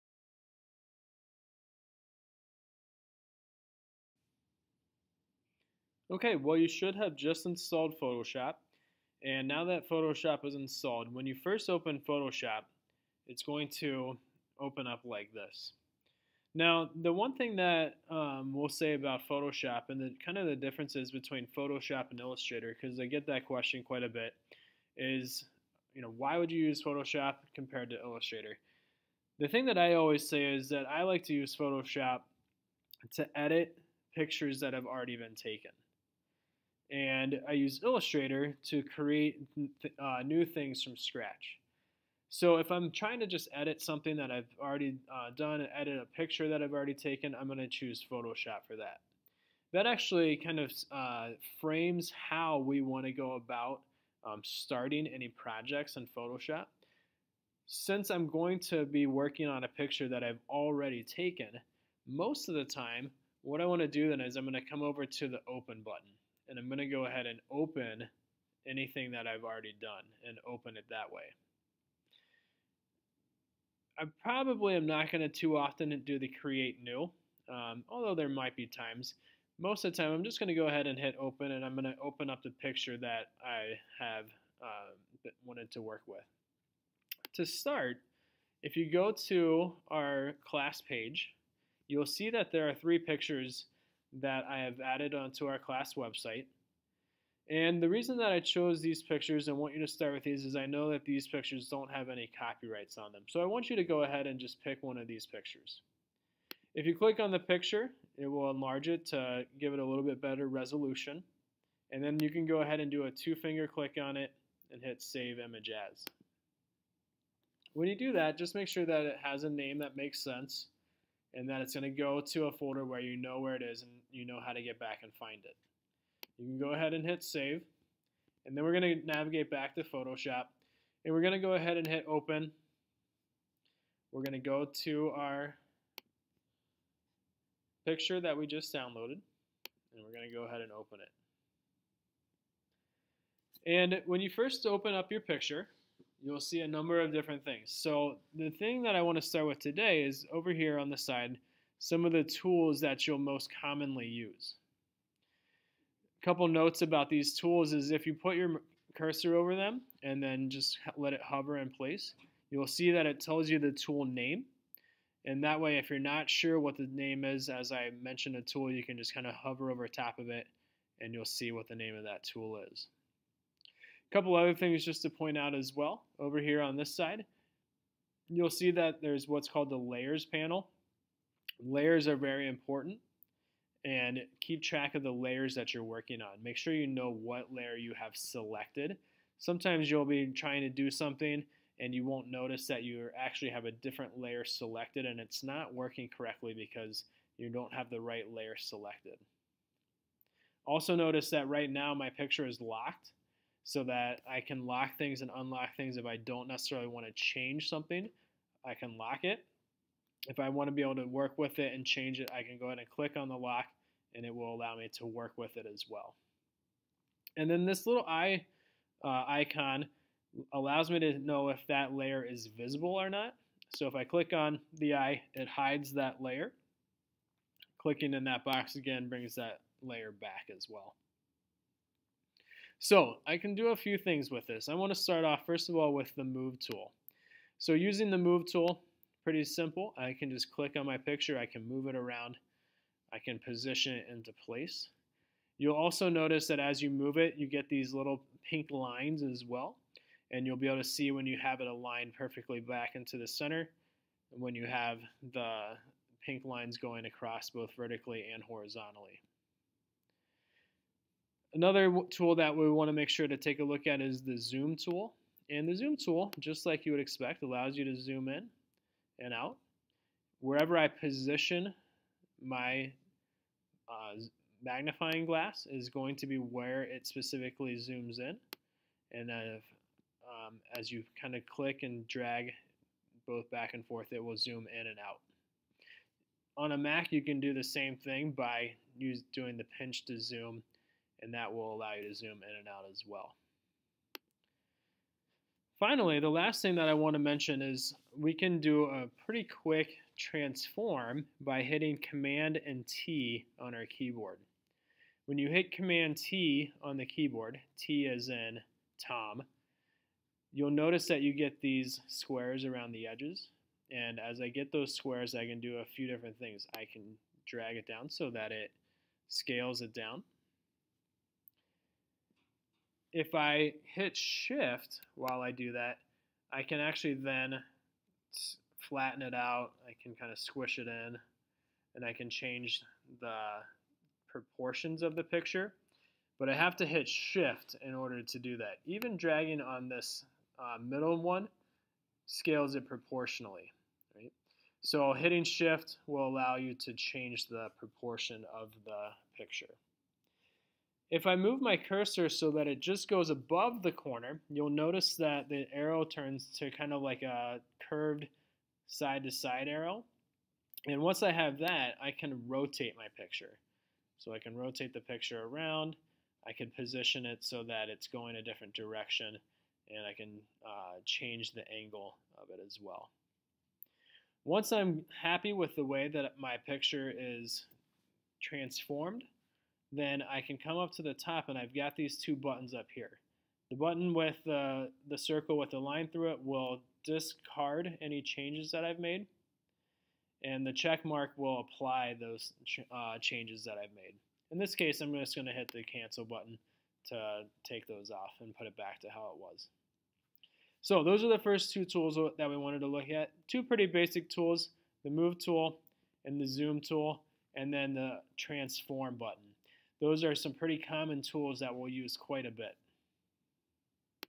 Photoshop Tutorial #1
In this video, we will learn how to use the move, zoom and transform tools.